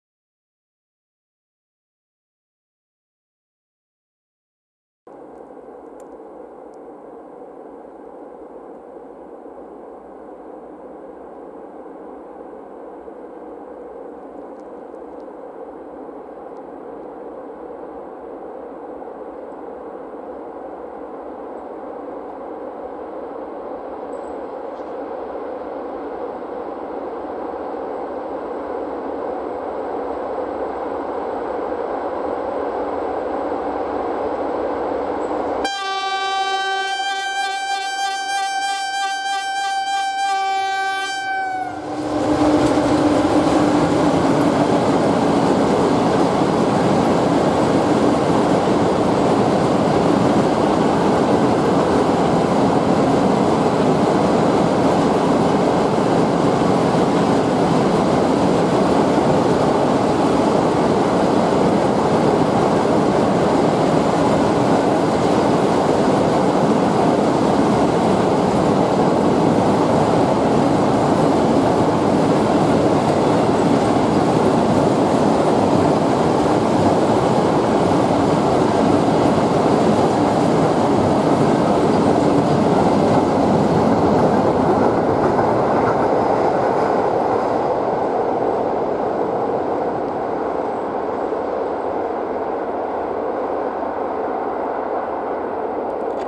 Miły maszynista EU07-240 | Nowa Sarzyna
OPIS
Siema!

Dziś film z pięknym Rp1 od siódemki ZIKu :)

Pozdrawiam załogę pociągu!
SKŁADY
0:00 zdjęcie
0:05 [Rp1] EU07-240 z samowyładowczymi i gruszkami w kierunku Stalowej Woli
nagrane za pomocą:
Panasonic HC-V180